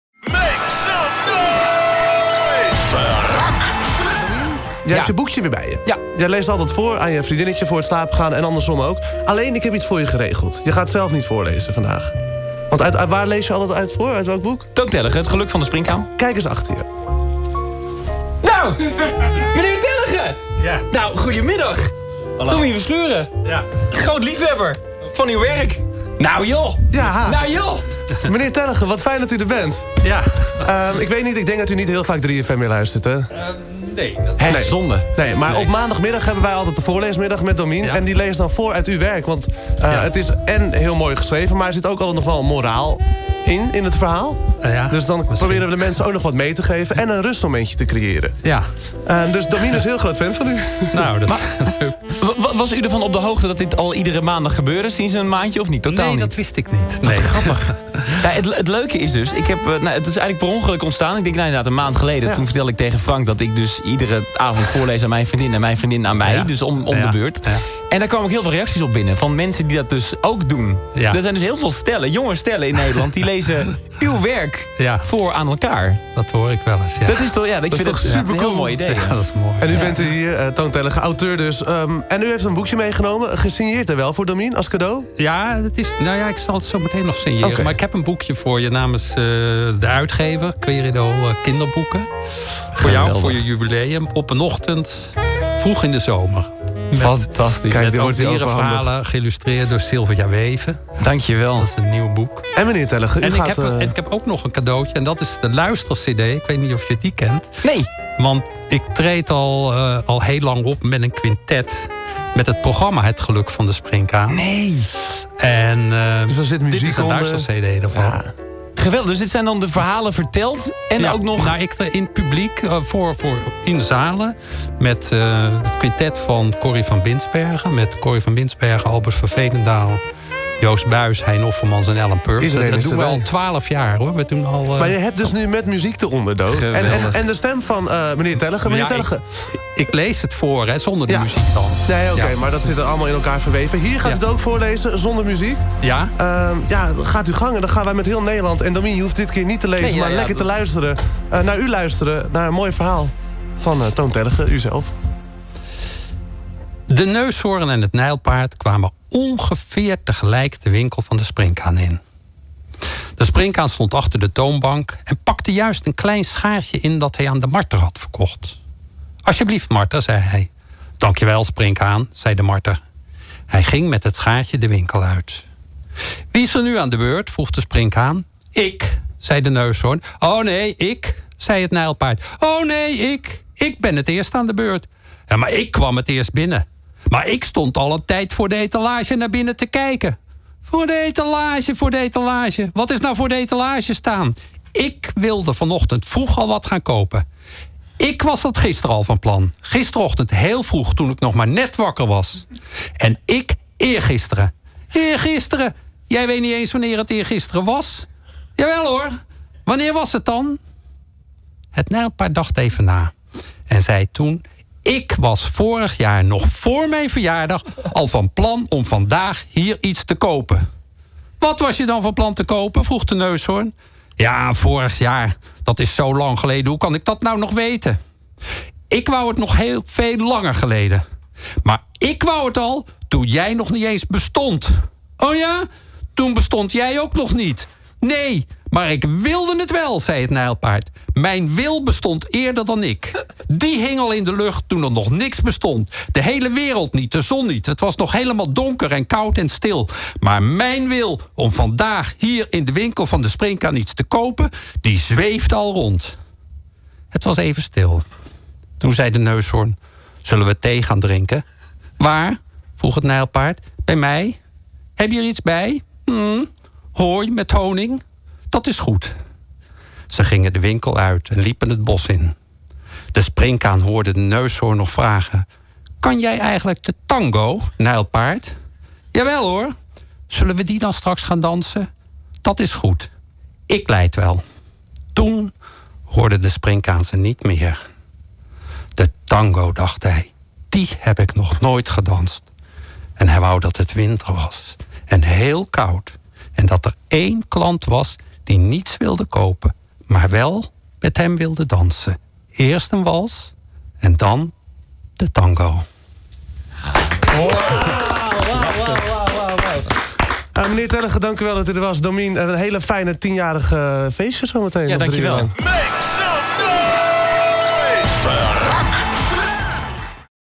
Domien leest niet voor; Toon Tellegen wel!
Elke maandag even voor 18:00 uur, leest Domien een verhaal van Toon Tellegen voor bij Frank